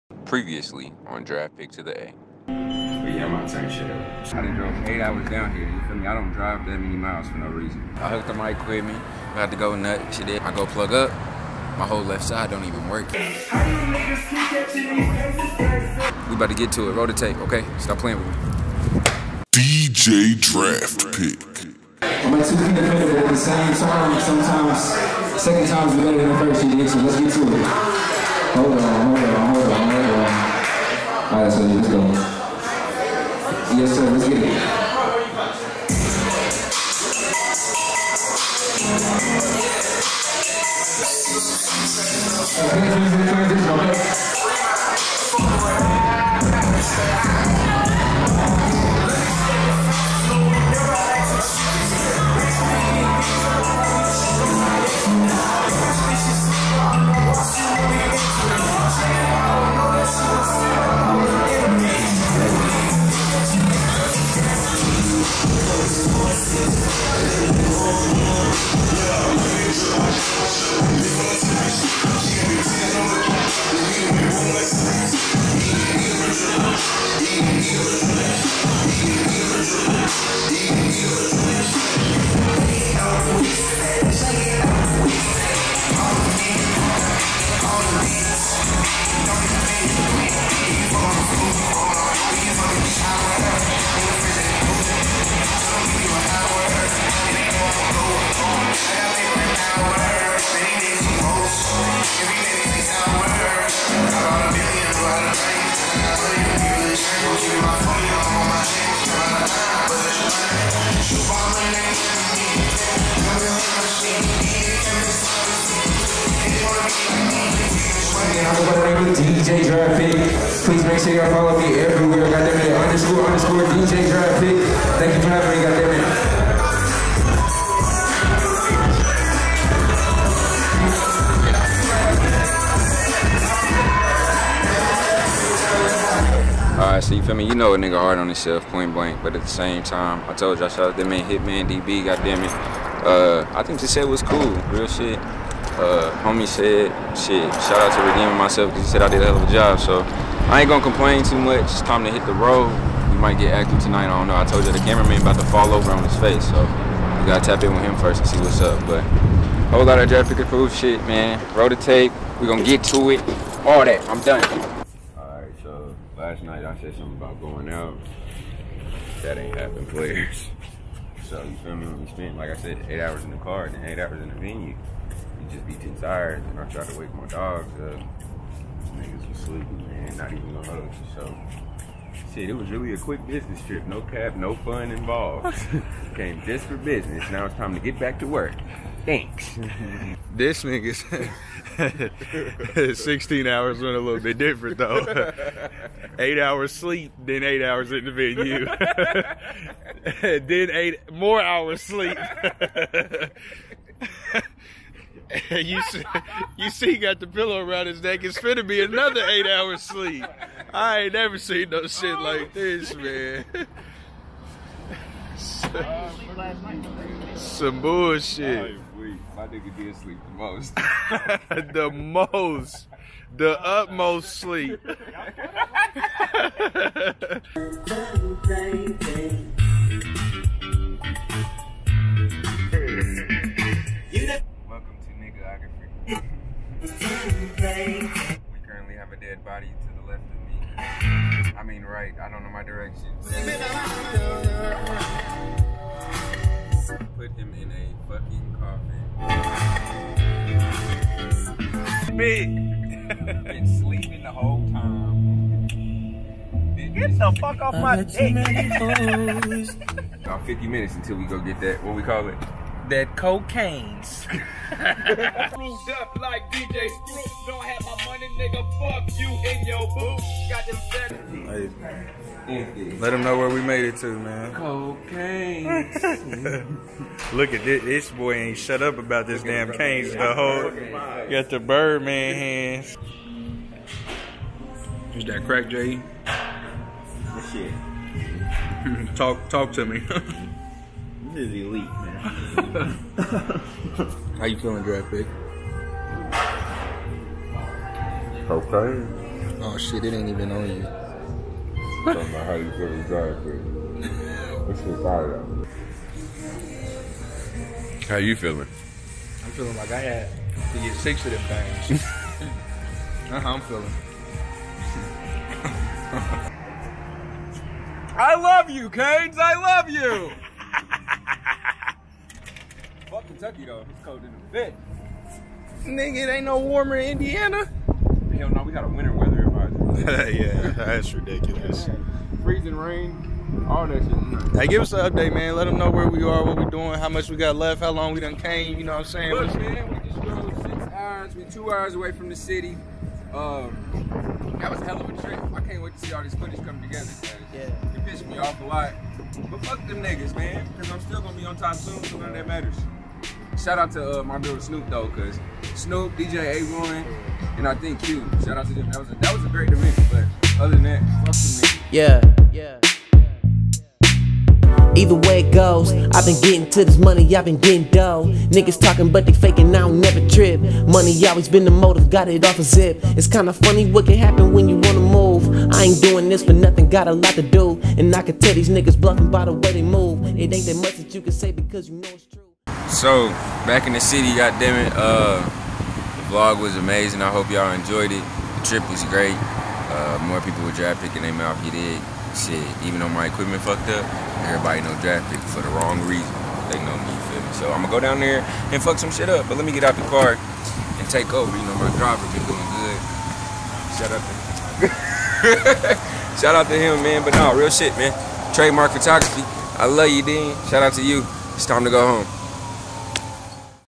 Results of Atlanta DJ Competition
Episode 3: Treydmark x Draft Pick Vlog Series

I told myself I wasn't leaving until someone heard me spin. Not the outcome I expected, but just more exposure to people I probably never would've crossed paths with. Turning all my losses into lessons and this experience definitely humbled me. Follow me in my third episode as Treydmarks catches footage of the outcome and our journey back to Nap